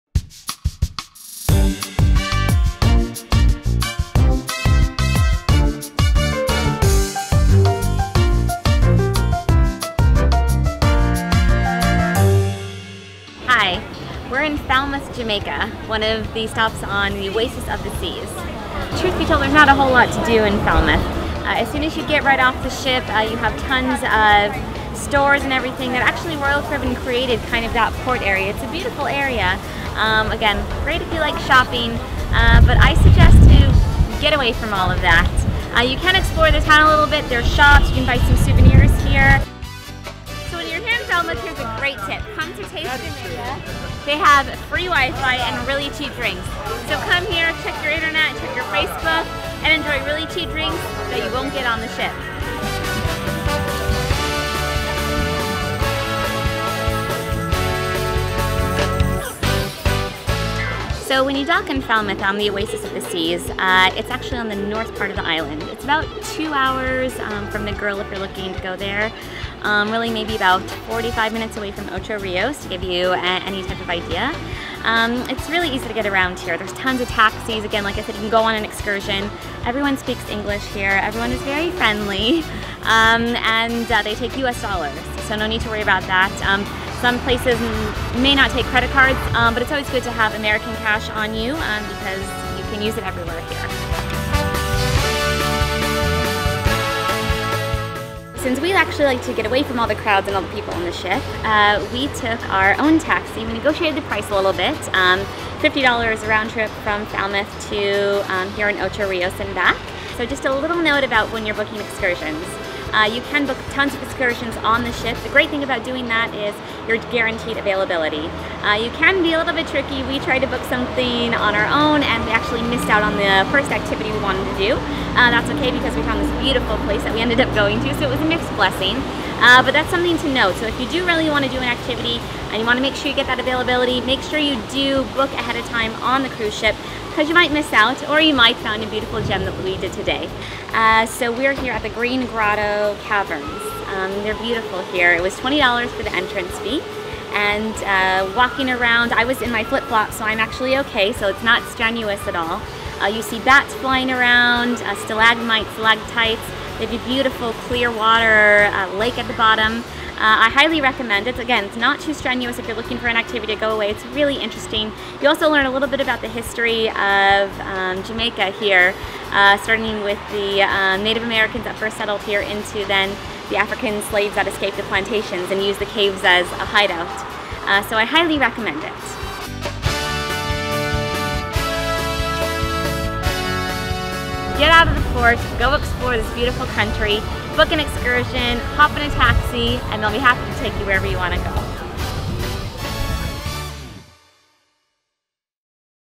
A Port Day in Falmouth
A stop in Falmouth, Jamaica while on Royal Caribbean's Oasis of the Seas!  A glimpse at some of the highlights of Falmouth and some great ways to spend your day.